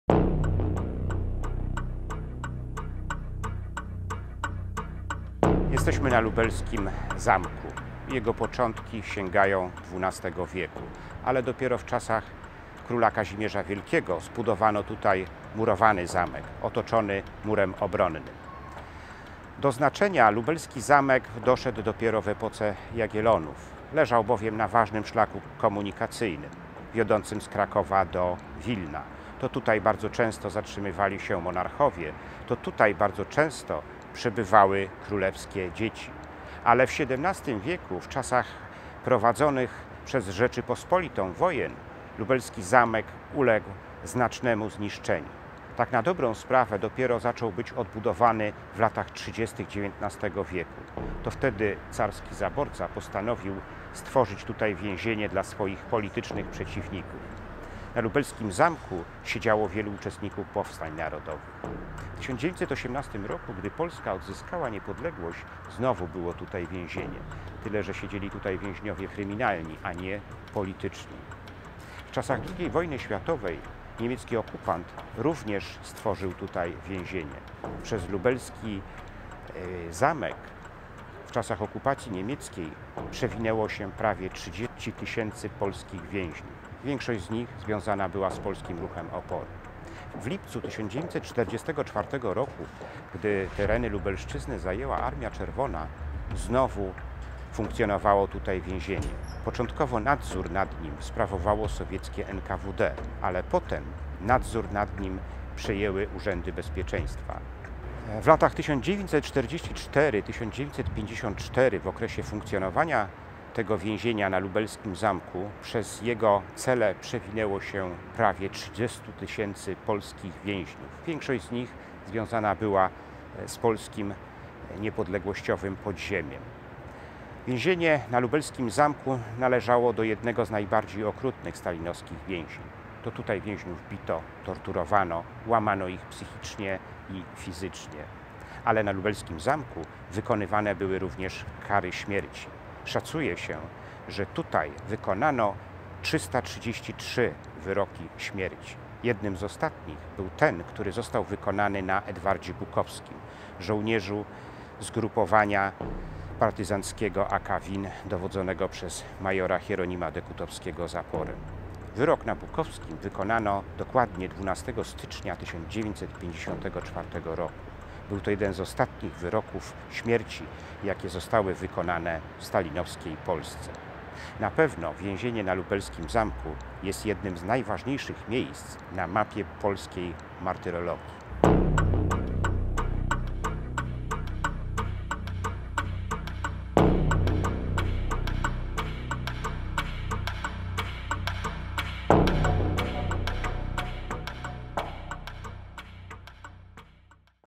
Miejsce stalinowskich kaźni w Lublinie
W tej poruszającej filmowej opowieści o najnowszej historii Polski opowiadam o słynnym lubelskim  miejscu kaźni, czyli Zamku Lubelskim. Więzienie na Lubelskim Zamku należało do jednego z najbardziej okrutnych stalinowskich więzień.  To tutaj więźniów bito, torturowano, łamano ich psychicznie i fizycznie. Wykonywano tutaj również kary śmierci. Historycy szacują, że na Lubelskim Zamku wykonano 333 wyroki śmierci. Jednym z ostatnich był ten wykonany na Edwardzie Bukowskim, żołnierzu zgrupowania partyzanckiego AK WiN, dowodzonego przez majora Hieronima Dekutowskiego "Zaporę". Wyrok na Bukowskim wykonano dokładnie 12 stycznia 1954 roku i był to jeden z ostatnich wyroków śmierci, wykonanych w stalinowskiej Polsce.